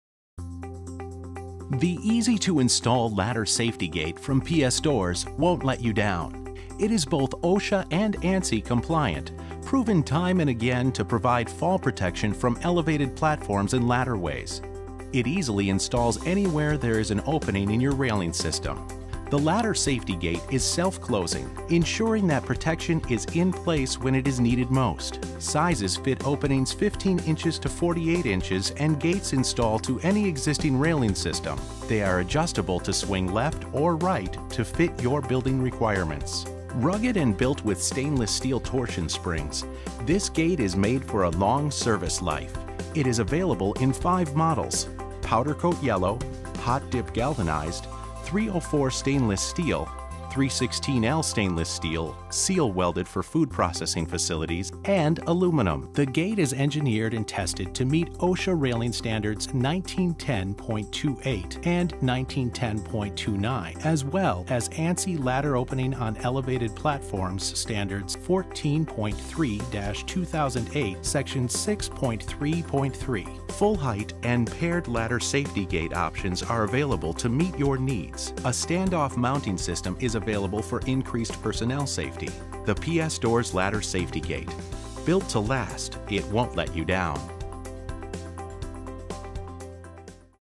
Ladder Safety Gate by PS DOORS
Your employees are your greatest asset, and every time they use a ladder in your facility, they put themselves at risk. Fortunately, the OSHA- and ANSI-compliant Ladder Safety Gate from PS DOORS provides proven protection, minimizing the chances of a fall on elevated platforms, mezzanines or ladder ways. This self-closing swing gate provides 42" top- and mid-rail protection to help ensure that your employees go home safe each night.

Available in five finishes: Powder Coat Yellow (PCY), Hot Dipped Galvanized (GAL), 304 Stainless Steel (SS), 316L Stainless Steel (SS-SW) and Aluminum (ALU).

Engineered and tested to comply with Ladder Safety Gate requirements in OSHA 1910.28 and 1910.29, as well as ANSI A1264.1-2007, A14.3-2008 and MH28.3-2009

[request_more